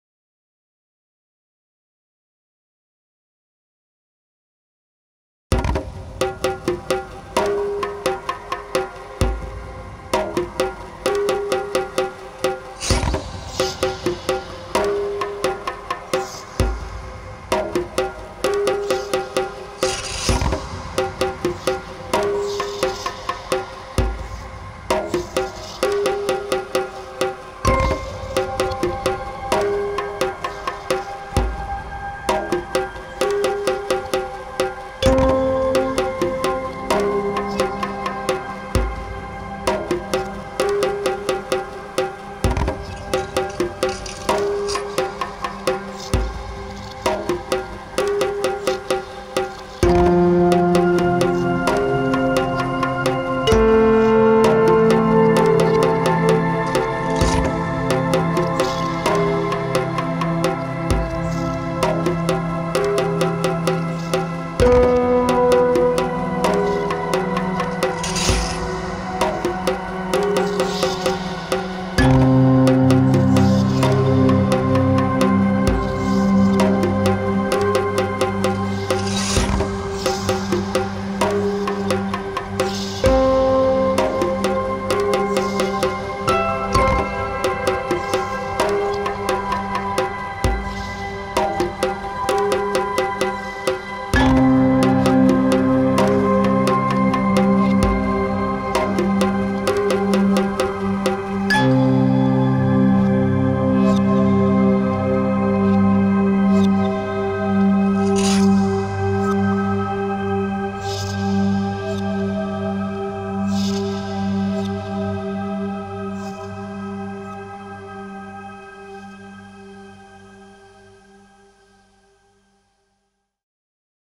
[2020/04/24] Indic Ambient Experiments (Akai Force + Kalimba + Chase Bliss Blooper)
Instruments:
 * Kalimba (with piezo microphone pickup)
 * Akai Force (for the sampled drum track)
 * effect pedals:
    + Chase Bliss Audio Blooper (used for slow tape looping effect)
    + TC Electronic HallOfFame 2 (a reverb)